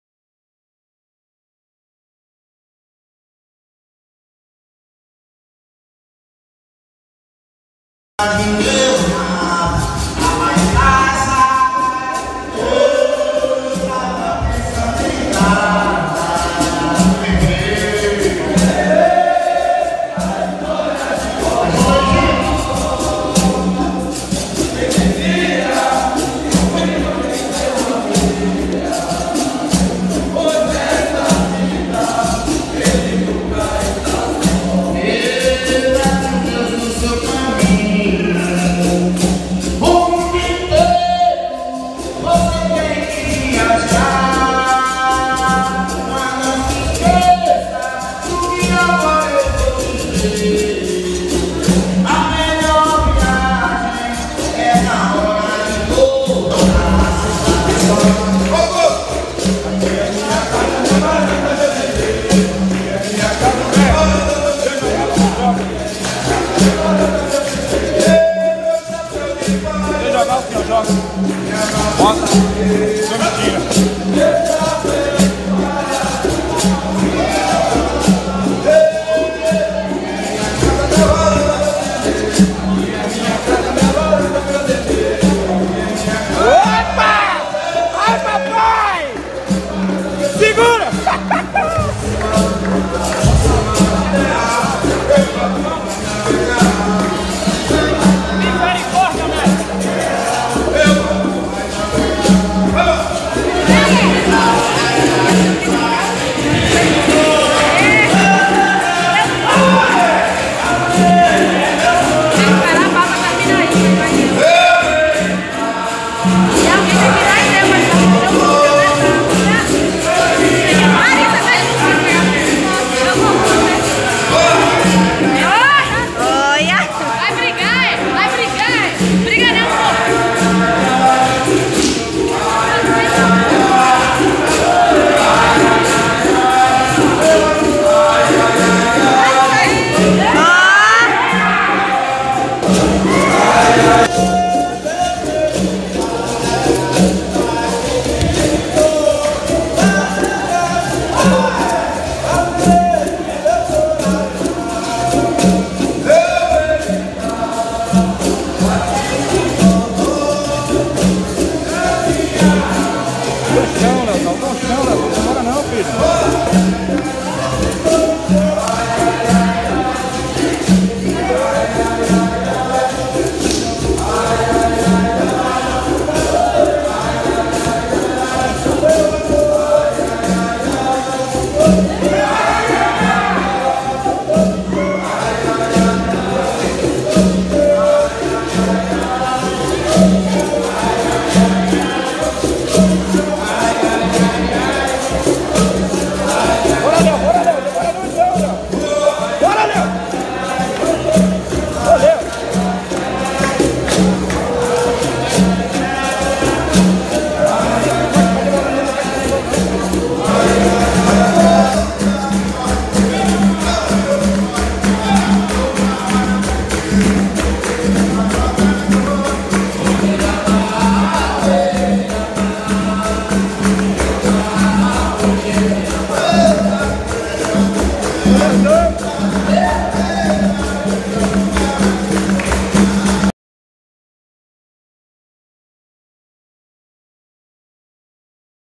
Me Lembrei Da História de Vovó! Mestrando Bailarino na Bahia.
Grupo Bamba Brasil - 2018

Jogo com Mestrando Bailarino, Batizado do Grupo Ginga Eunápolis Mestre Eudóchio em Eunápolis - BA

Angra dos Reis - Rj
Mestre Mamute
Mestre Espaço
Mestrando Bailarino